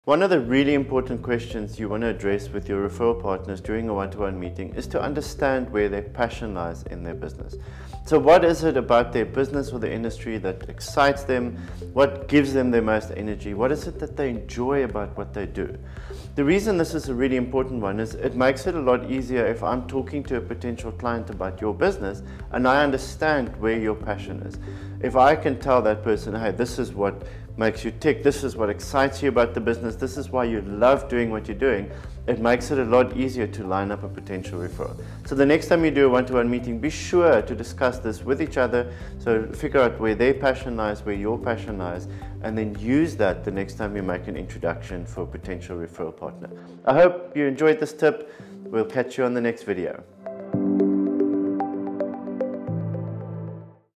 Networking tip - What’s your passion?
When you can explain why you’re so passionate about your business – what excites you about what you do – it makes lining up a potential referral so much easier for your referral partner. Watch here to see why.